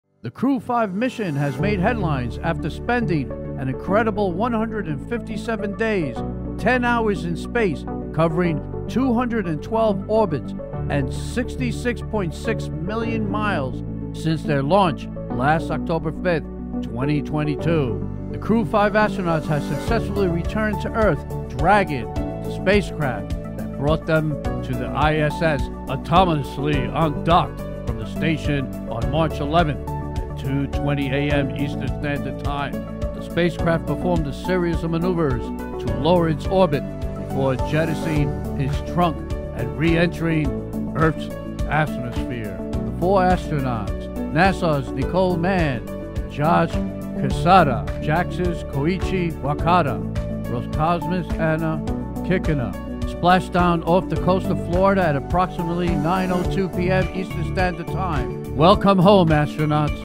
"Mission Accomplished! Crew-5 Astronauts Successfully Return to Earth!"
The Crew-5 mission has made headlines. After spending an incredible 157 days, 10 hours in space, covering 2,512 orbits and 66.6 million miles since their launch last October 5, the Crew-5 astronauts have successfully returned to Earth. Dragon, the spacecraft that brought them to the ISS, autonomously undocked from the station on March 11th at 2:20 a.m. ET.

 The spacecraft performed a series of maneuvers to lower its orbit before jettisoning its trunk and re-entering Earth's atmosphere. The four astronauts, NASA's Nicole Mann and Josh Cassada, JAXA's Koichi Wakata, and Roscosmos' Anna Kikina, splashed down off the coast of Florida at approximately 9:02 p.m. ET. This mission marks a significant achievement for international space cooperation and sets the stage for future endeavors in space exploration. We congratulate the Crew-5 astronauts on their safe and successful return home!

✅ Hosted by Antonio Saillant

Video Credit: Space X
✅  Courtesy of Space X

✅ This is an ideal attribution because it includes the:
* Title: Crew-5 Mission | Return